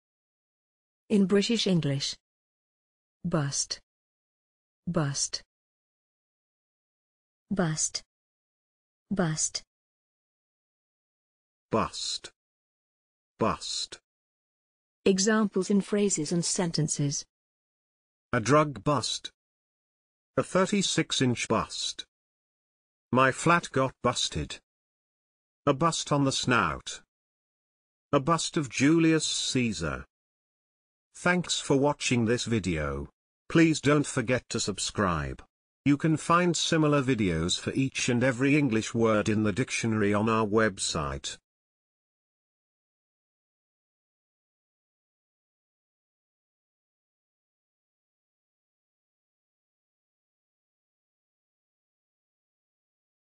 bust - pronunciation + Examples in sentences and phrases
This is the pronunciation of bust in British English + a few examples of its pronunciation in phrases and sentences.